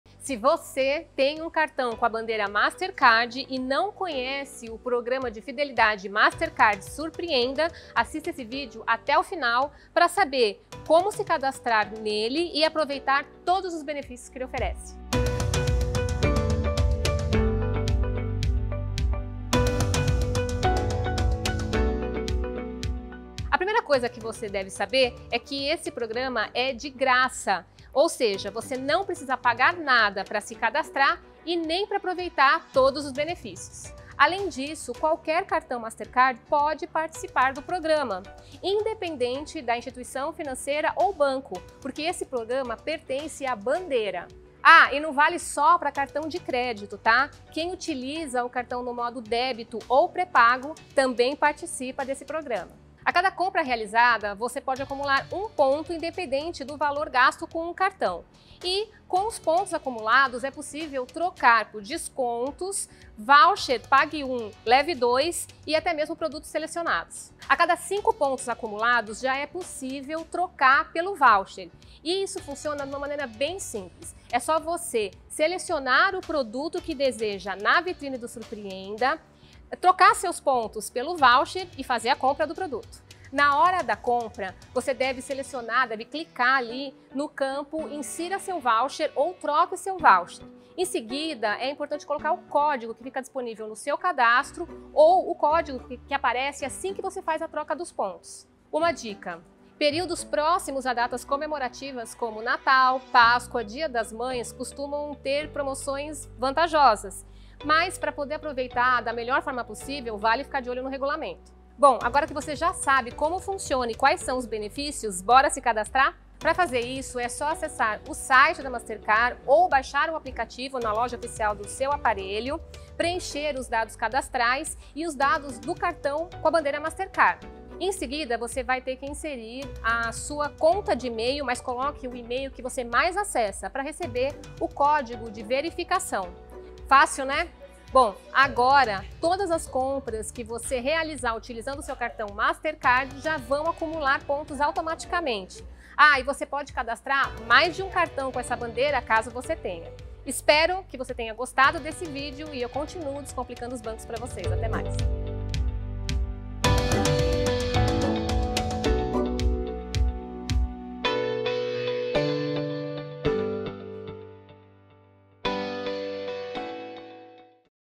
COMO FUNCIONA O MASTERCARD SURPREENDA - Foregon.com
Se você tem um cartão Mastercard e ainda não se cadastrou no programa de benefícios Mastercard Surpreenda, então assista a esse vídeo que vamos te explicar como funciona e como aproveitar todos os benefícios desse programa.